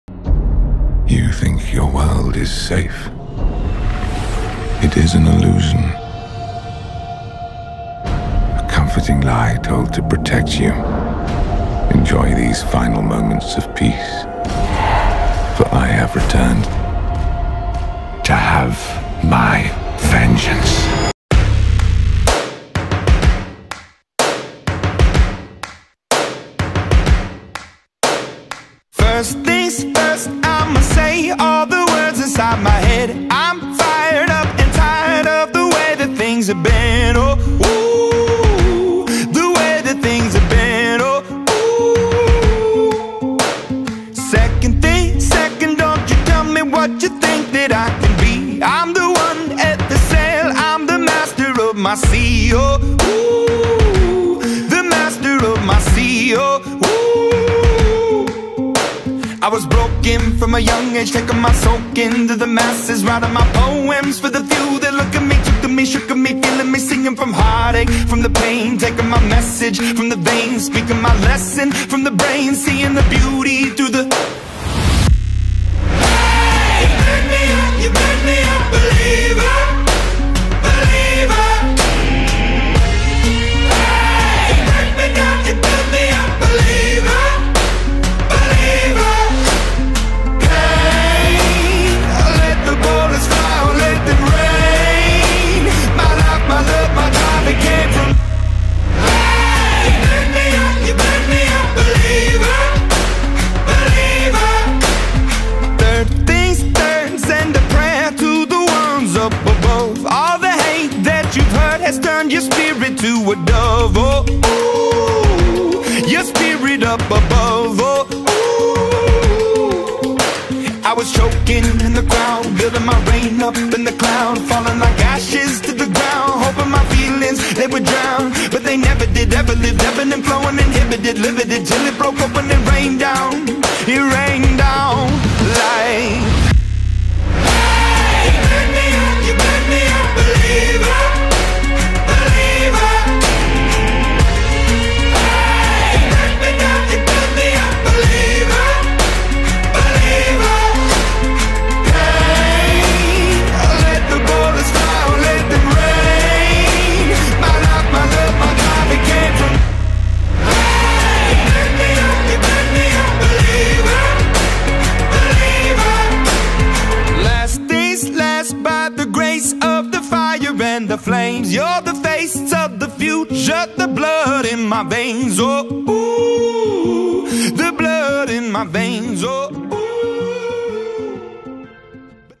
Oklahoma Sooners Playoff Hype | 2017-18
Congratulations to the Oklahoma Sooners on a fantastic season and congratulations to Baker Mayfield on a historic career!

Opening Voiceover | "TCU Football Hype Video | Vengeance"